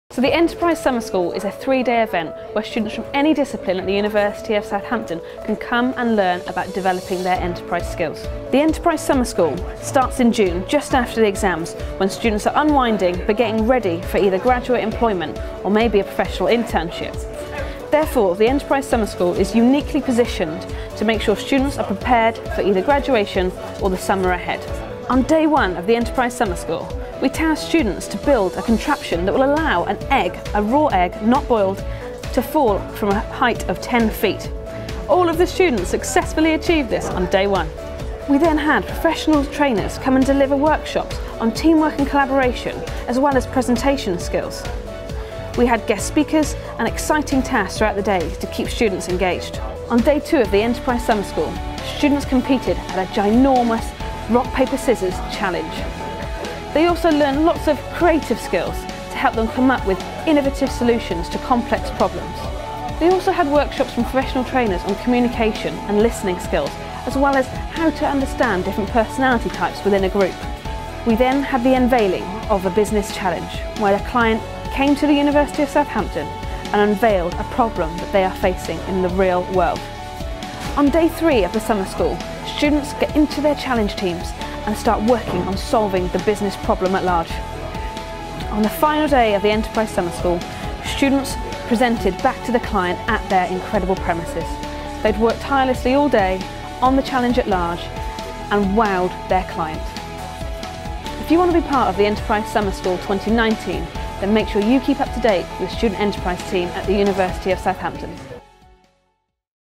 Enterprise Summer School 2018
The Enterprise Summer School is a three day professional training programme full of workshops and exciting challenges that enhance student's enterprising skill sets and test their ability to find a solution to a real commercial problem.